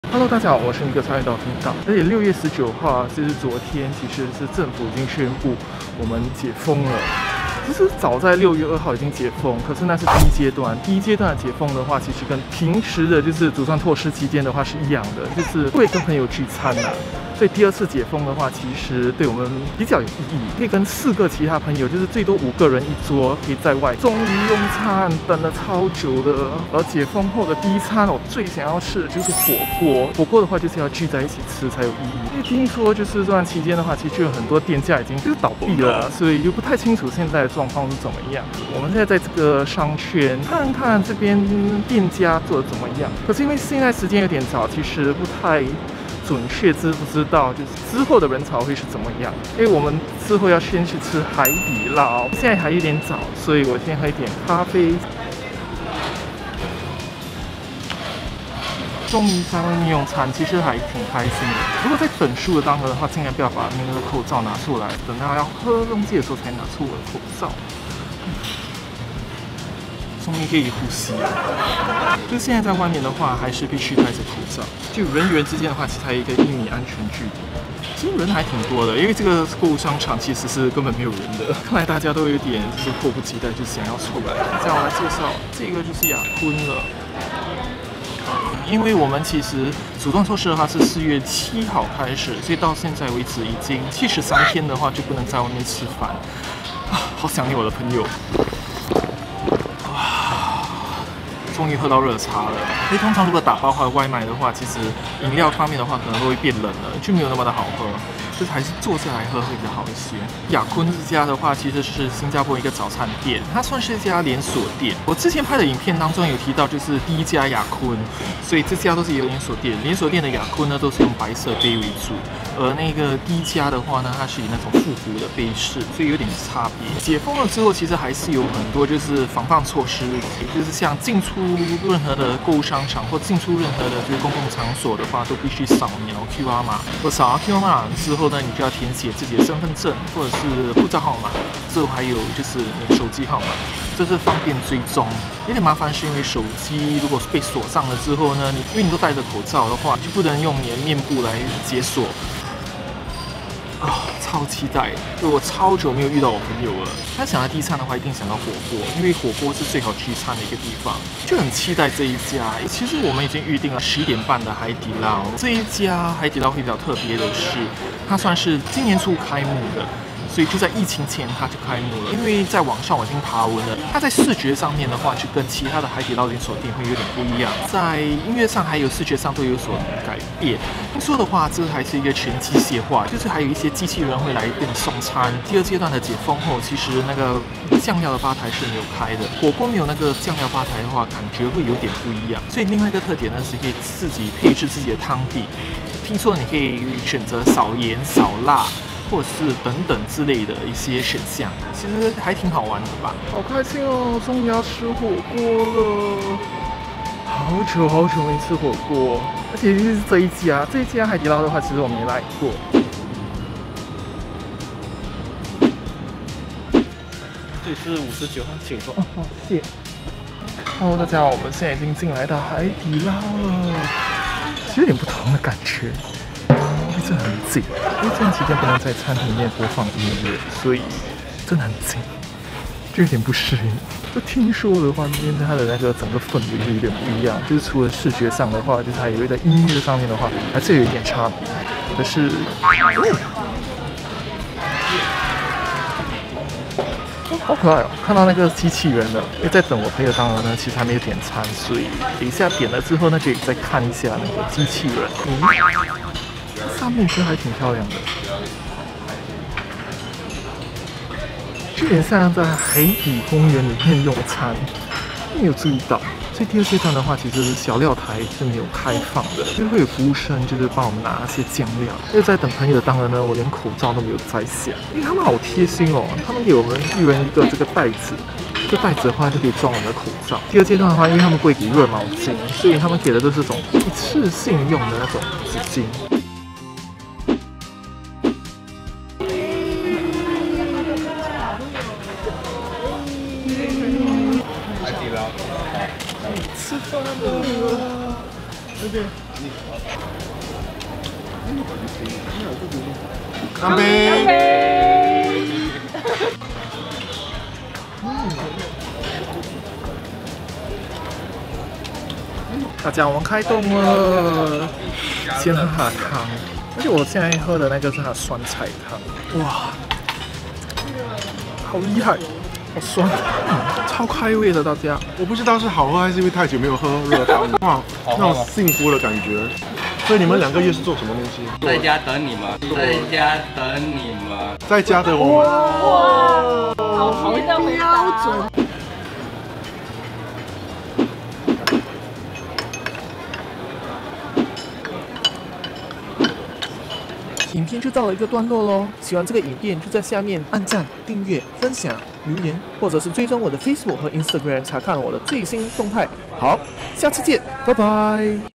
新加坡解封了！直擊唯一一家海底撈機器人火鍋店！| Stormscape
新加坡在6月19日結束了長達73天的“阻斷措施”，在第二階段的解封，各個餐廳也開放在店內用餐。終於可以和朋友聚餐，但一桌限制最多5個人。

第一次來到全新加坡唯一一家機器人海底撈火鍋店，因為第二階段解封的關係，不能在店內播放音樂所以少了一點點氣氛，而小料台也是關閉的。
但能和朋友聚餐就是幸福的，其餘的都不重要了。